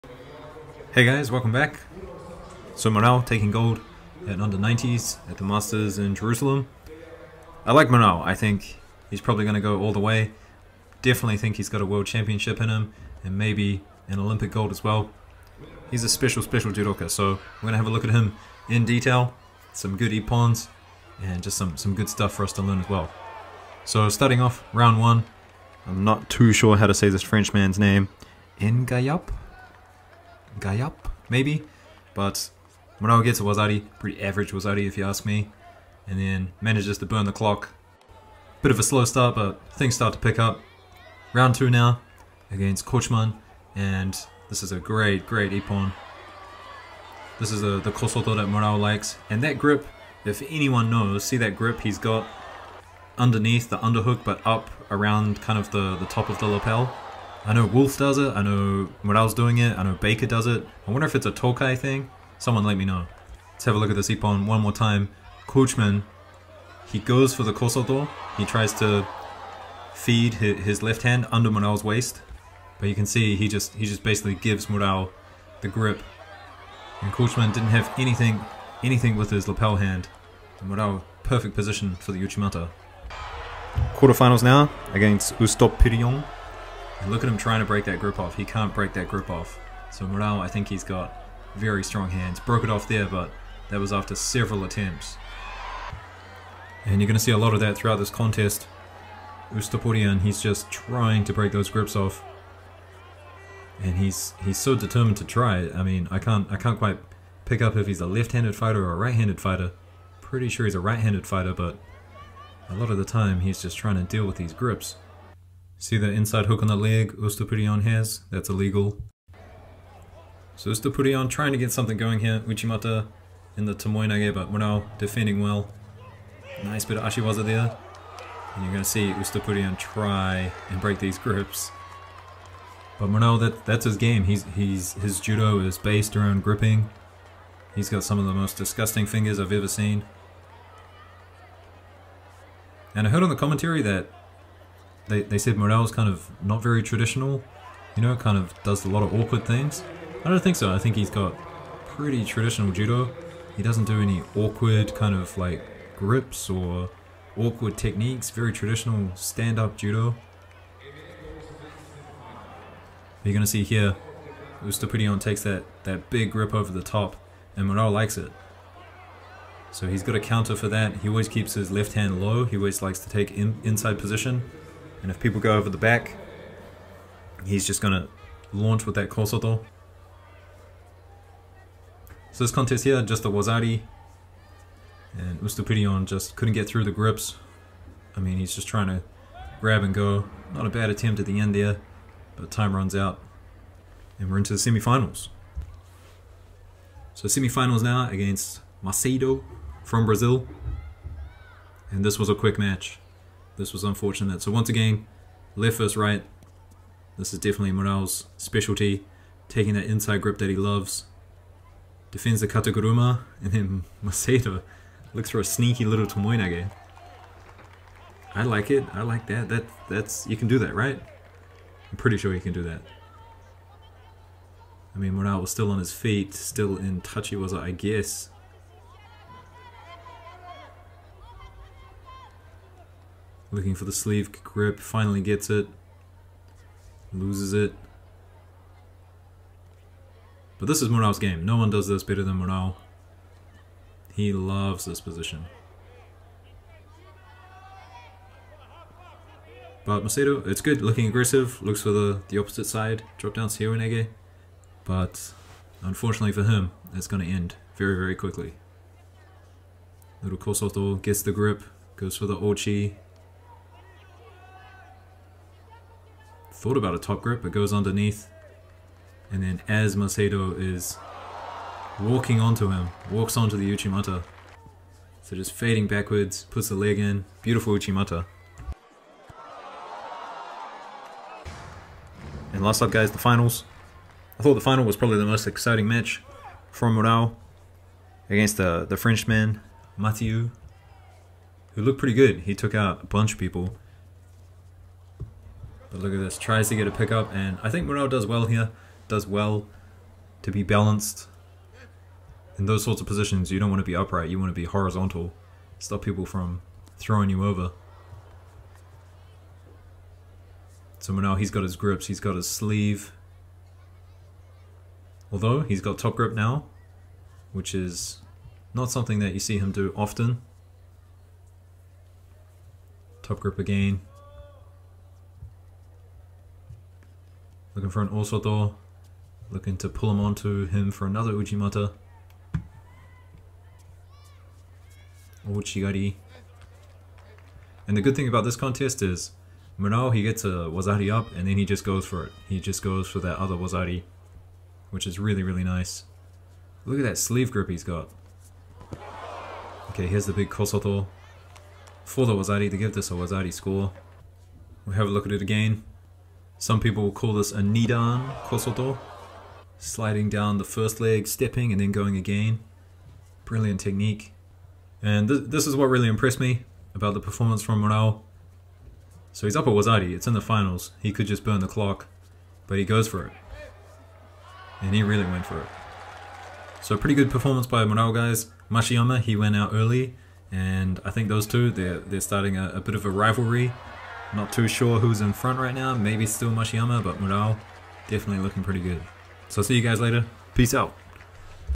村尾 優勝 Murao Takes Gold at Judo Masters 2022
Pretty solid performance by Murao. I still want to seem him vs Bekauri since it's been a while since they last fought.

Awesome data on all your favorite judoka!

- Best Ippons.
- Tournament Recaps.